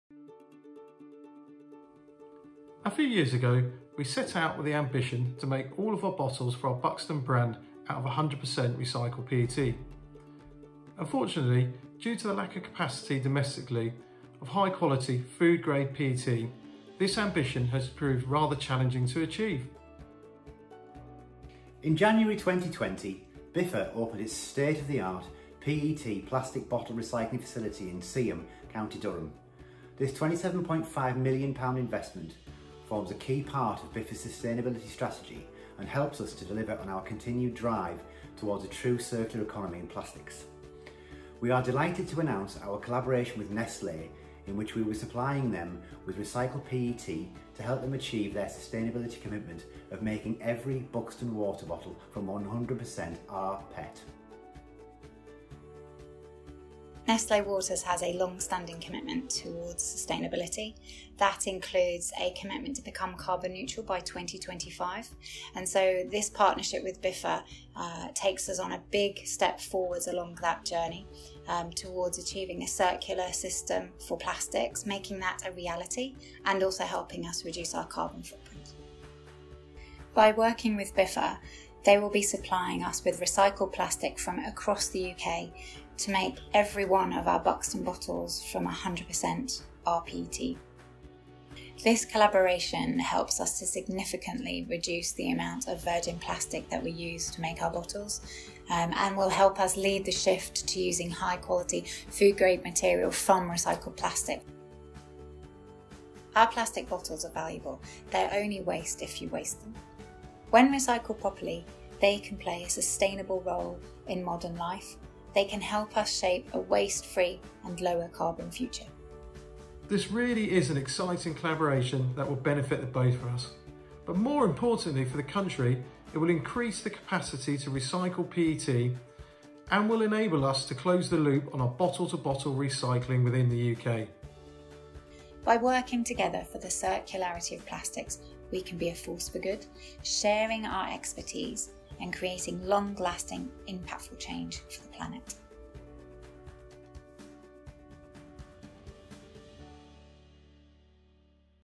Biffa and Nestlé Waters UK partner to deliver 100% rPET made from British recycled plastic
Biffa will be supporting Nestlé Waters UK to achieve its commitment of making every Buxton bottle from 100% recycled PET (recycled polyethylene terephthalate or rPET) by 2021, by collecting recyclable PET bottles and reprocessing them here in the UK into rPET.